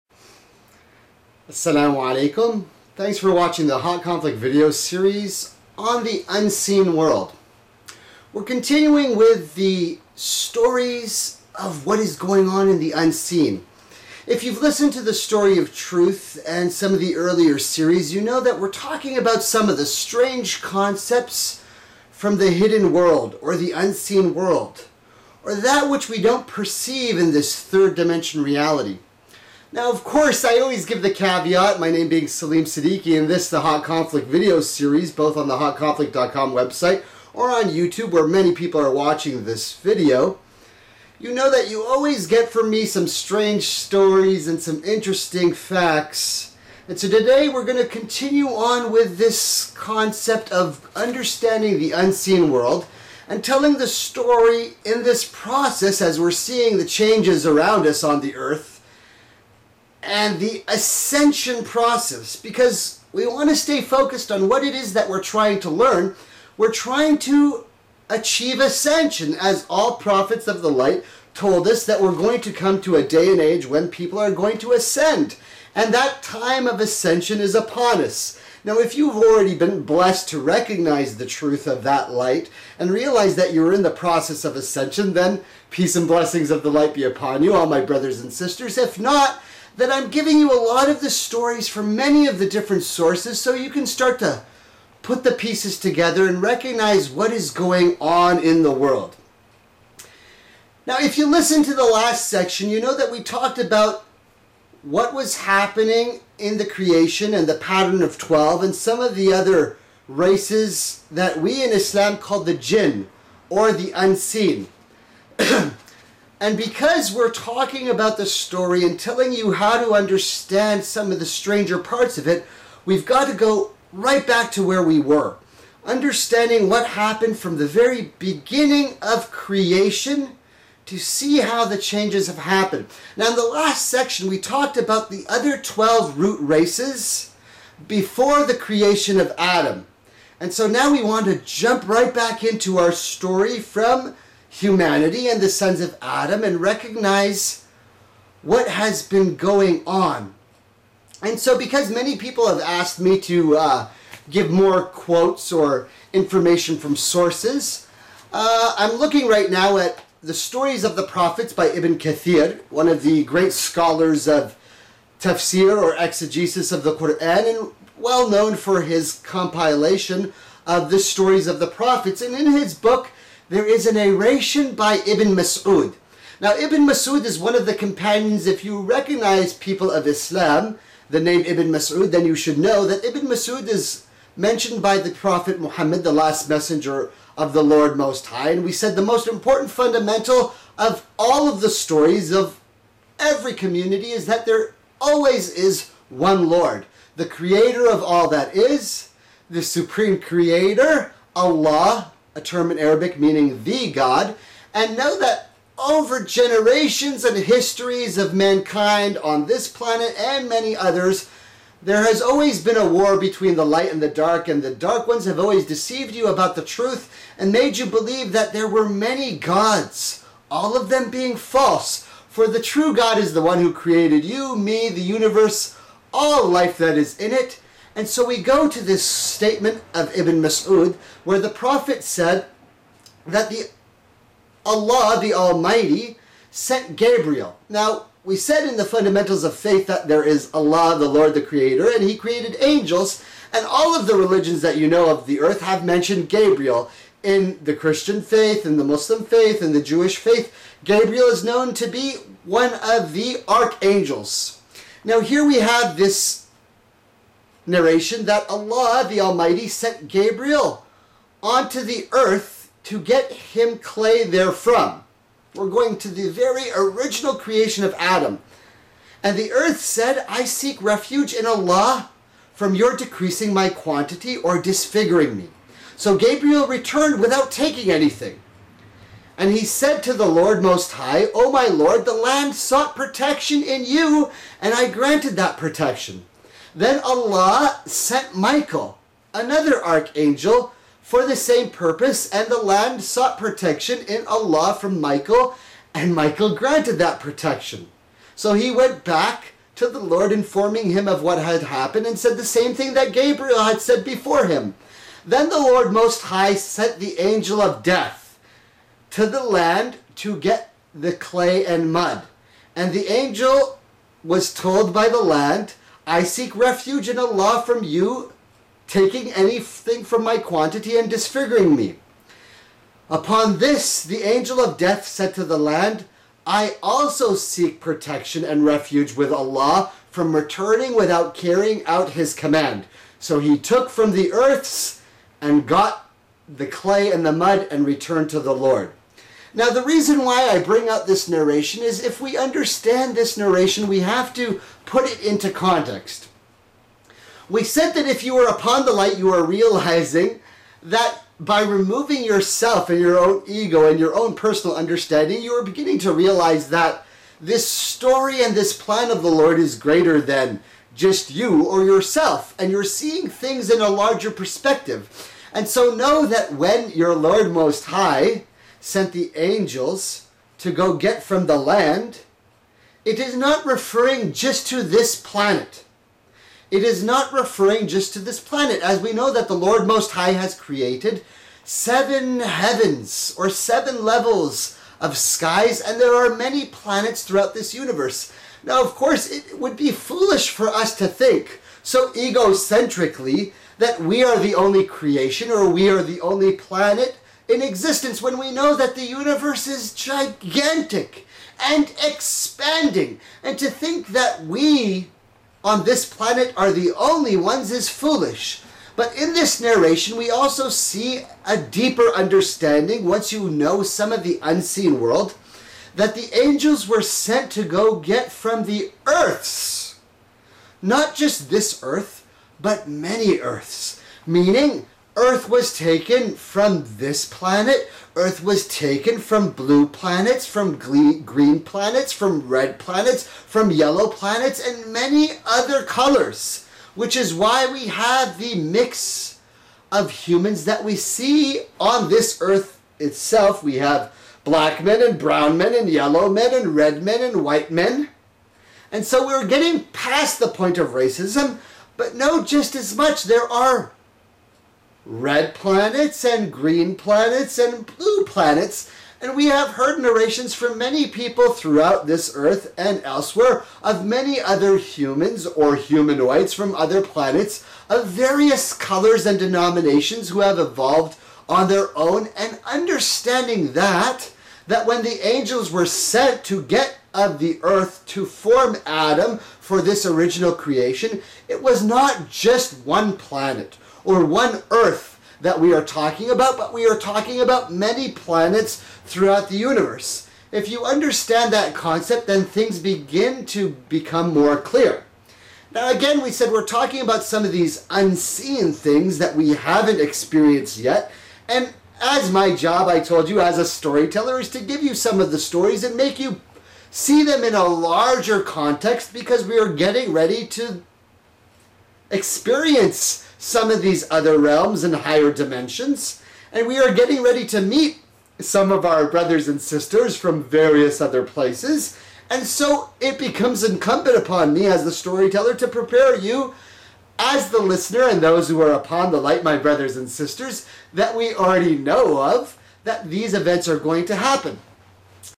Unseen pt 11 Arch Angels Gabriel Michael Death Blue Red Green Planets Creation Adam
LIGHT WORKER STARSEED PATH Saleem Siddiqui discusses the advent of the final messenger of the Prime Creator to mankind on this Planet. All prophets are of truth bring the Light of the Lord in the Spoken word. Listen to the whole story to hear the true tale of our race and this earth. Are you looking for the fractal patter or the thread of life? Do you see the weave in the Matrix? We are all realizing that the world around us is changing, are you searching for something? Keep asking questions and answers will present themselves. SEEK and YE Shall FIND. Salam Peace. Islam = submitting to the Will of the Lord Most High, the Light.